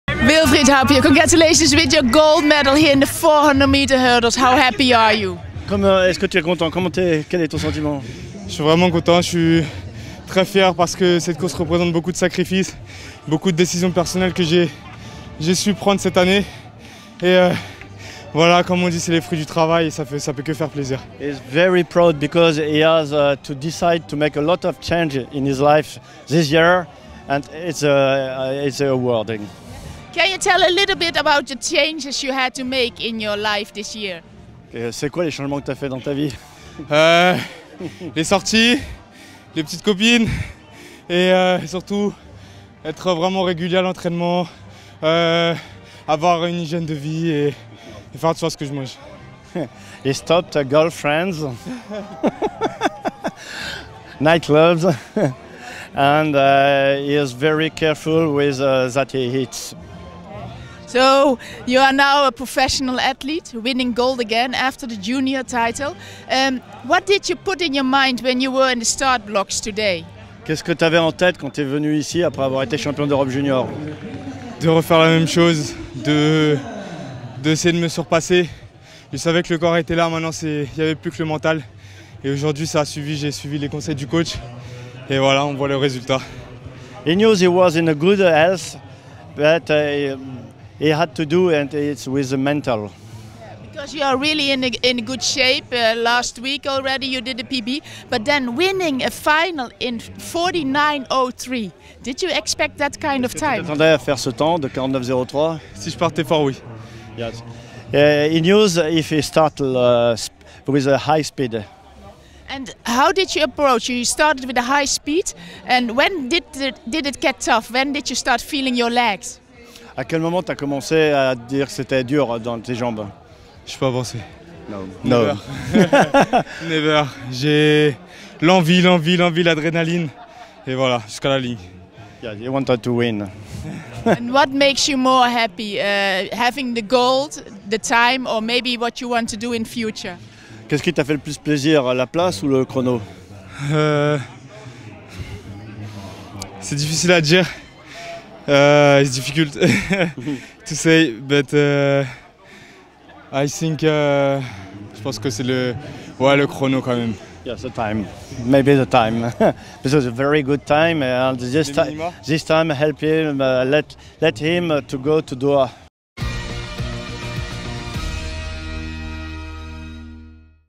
Wilfried Happio (FRA) after winning Gold in the 400m Hurdles
Wilfried Happio (FRA) after winning Gold in the 400m Hurdles at the European Athletics U23 Championships in Gävle